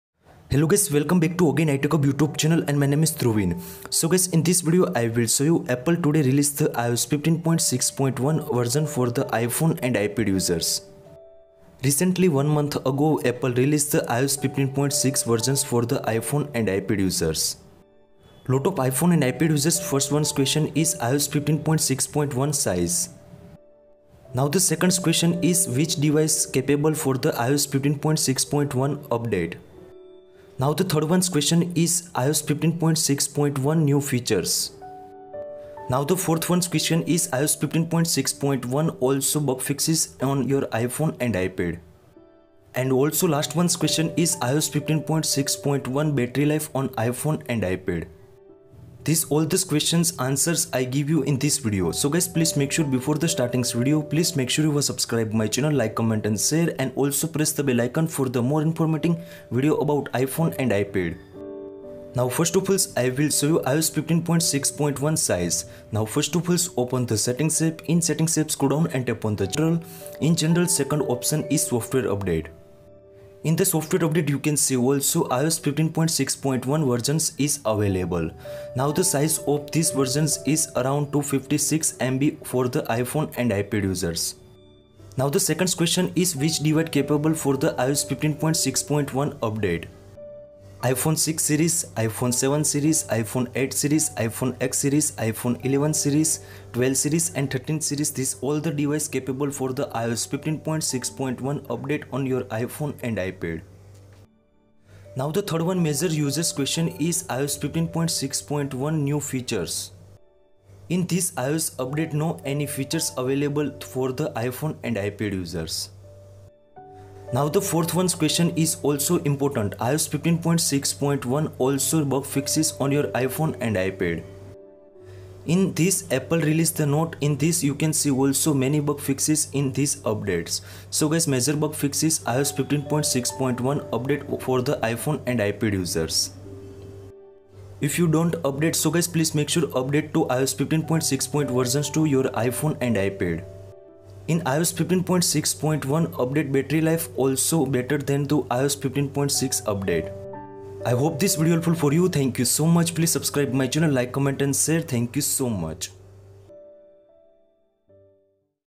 iOS 15.6.1 iPhone 7 Plus (FULL REVIEW)
1. iOS 15.6.1 Size
2. iOS 15.6.1 Which Device Capable
3. iOS 15.6.1 New Features
4. iOS 15.6.1 Bug Fixes
5. iOS 15.6.1 Battery Drain